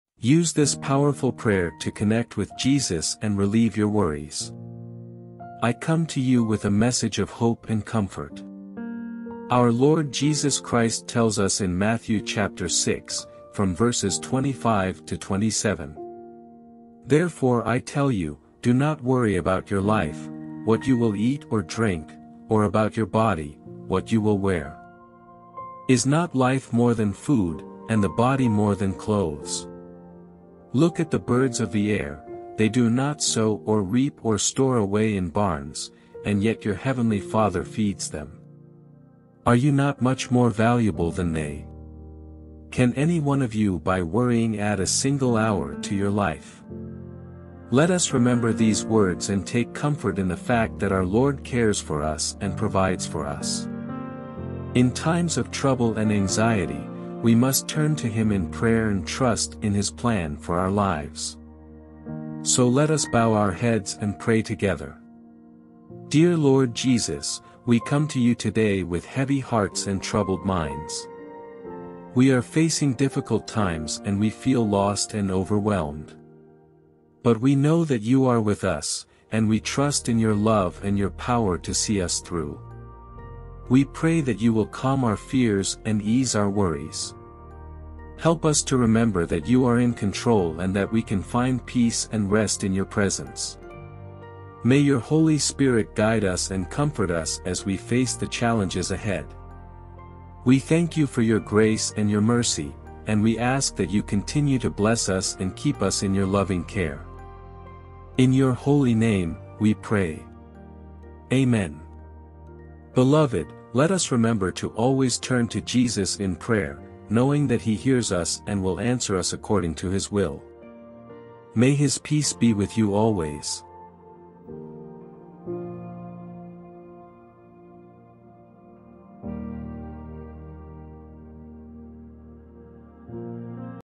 Use this powerful prayer to connect with Jesus and relieve your worries
Use this powerful prayer to connect with Jesus and relieve your worries. It cites Matthew 6:25–27 to remind us not to worry about our lives, as God always provides for us. In the prayer, we ask our Lord Jesus to calm our fears, ease worries, guide and comfort us during difficult times, and to bless and keep us in loving care. May our God richly bless you.